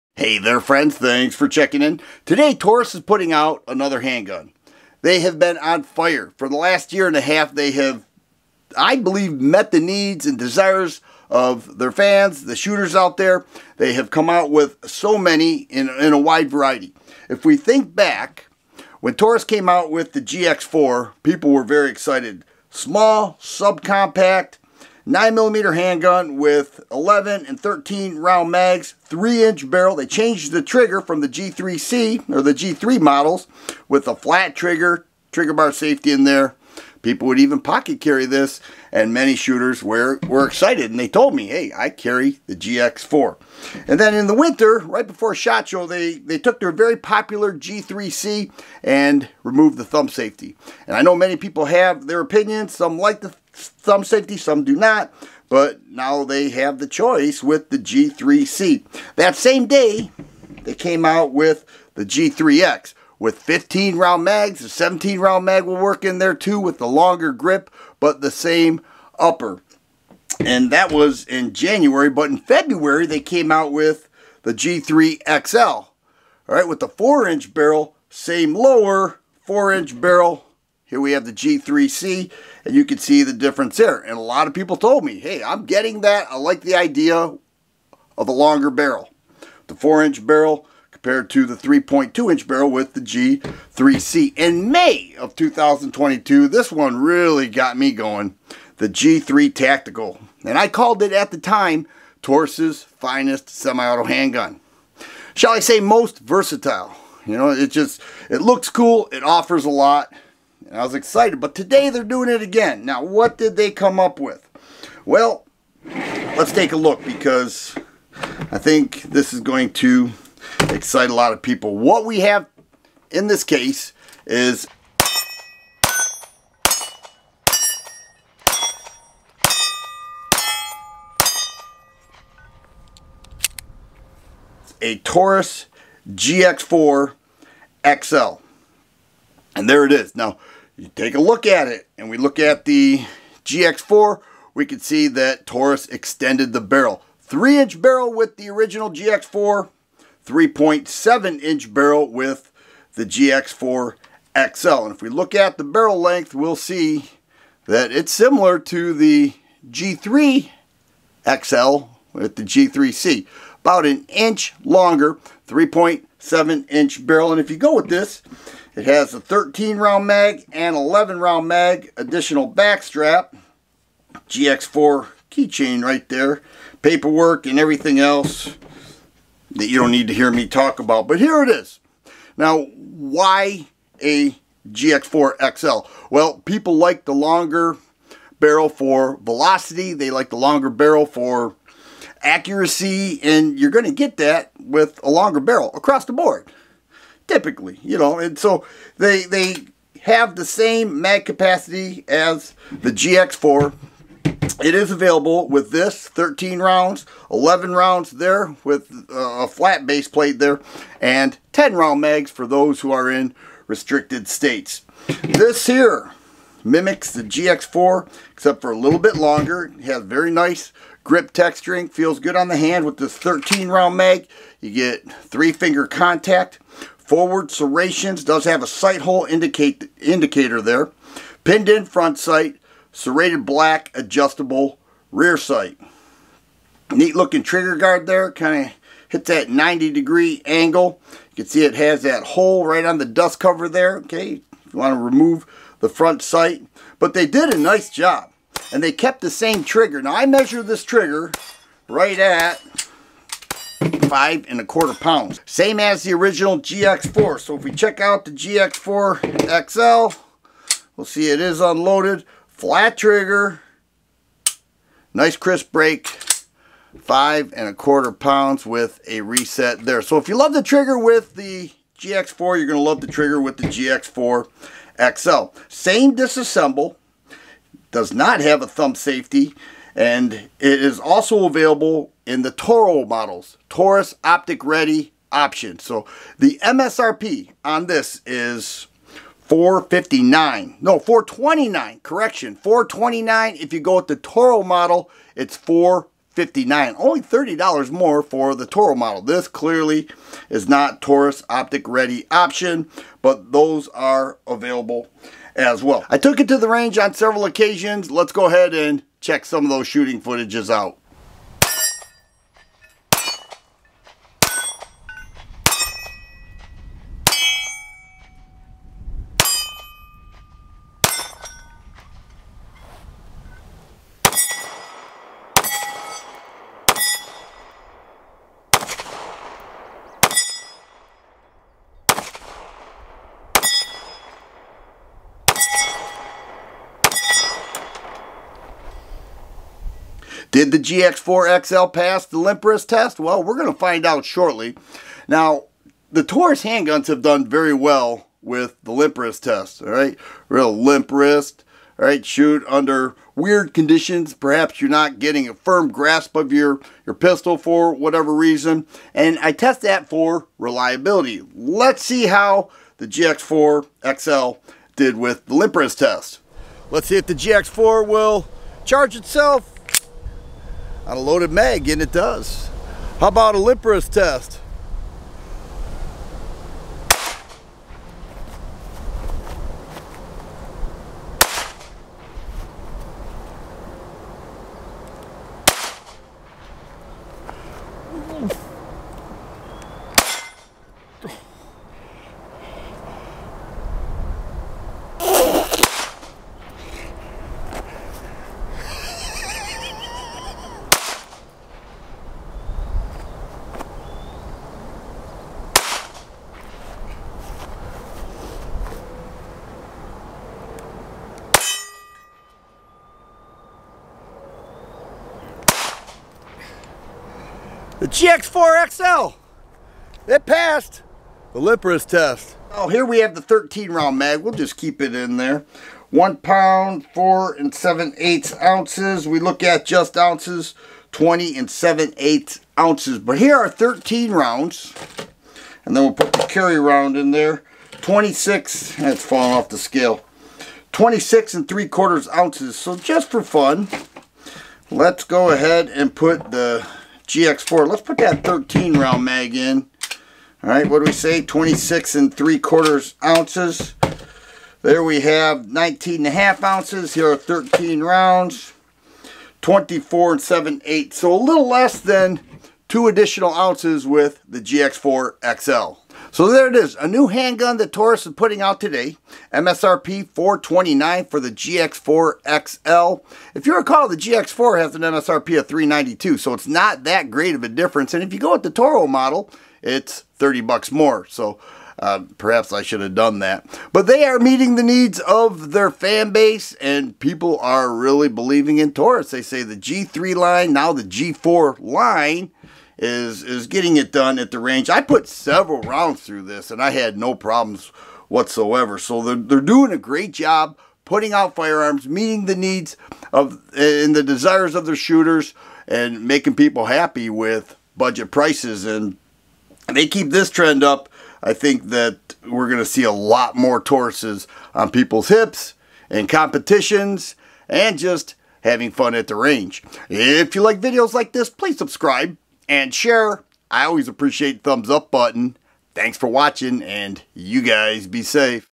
NEW Taurus Handgun "GX4XL" - TheFirearmGuy
NEW Taurus Handgun "GX4XL" Taurus is back at it with another fine handgun. What are your thoughts?